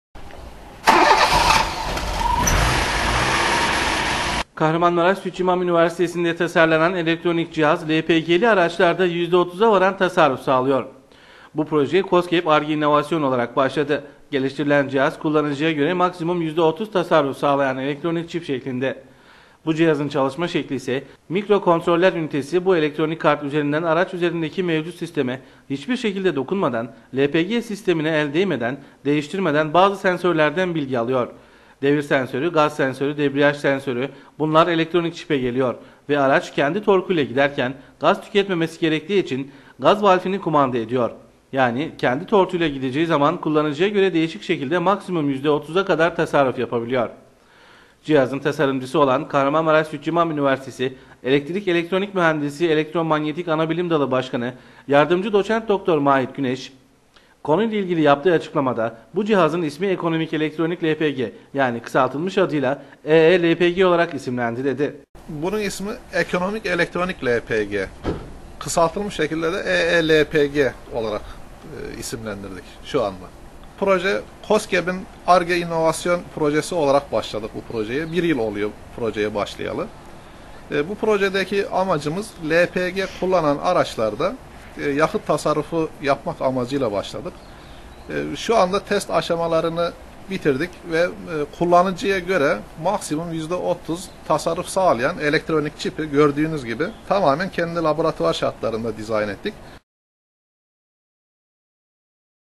YAKITTA TASARRUF SAĞLAYAN EE LPG
KAHRAMANMARAŞ SÜTÇÜ İMAM ÜNİVERSİTESİNDE (KSÜ)TASARLANAN, ELEKTRONİK CİHAZ LPG 'Lİ ARAÇLARDA YÜZDE 30'A VARAN TASARRUF SAĞLIYOR.